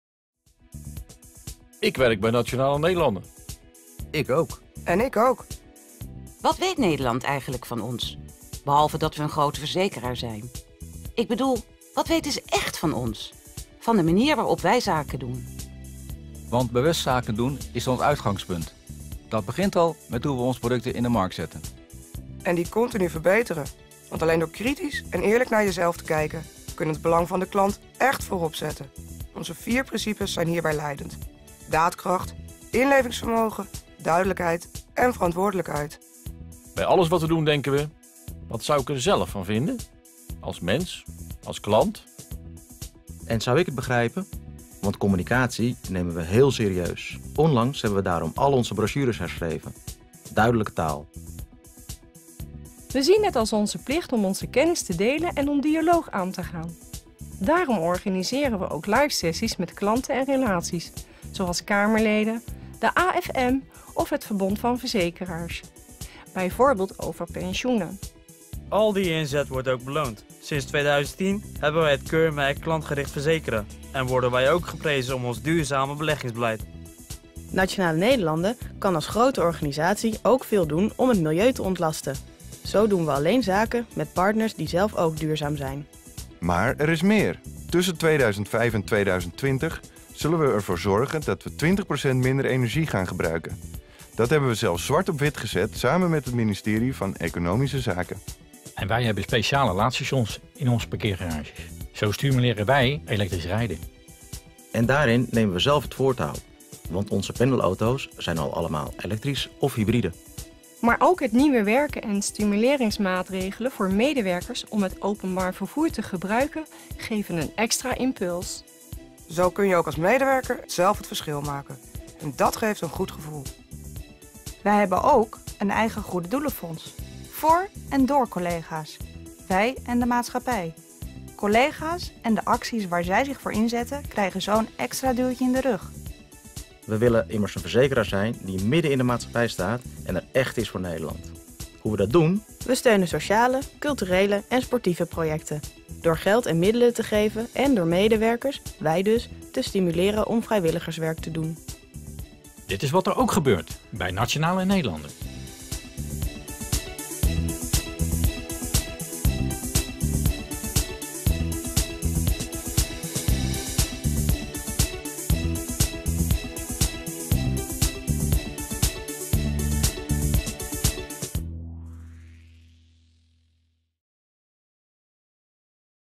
Nationale-Nederlanden opereert verantwoord, milieubewust en betrokken
Nationale-Nederlanden is actief betrokken bij de Nederlandse samenleving en ziet het ook als haar taak om er voor Nederland te zijn.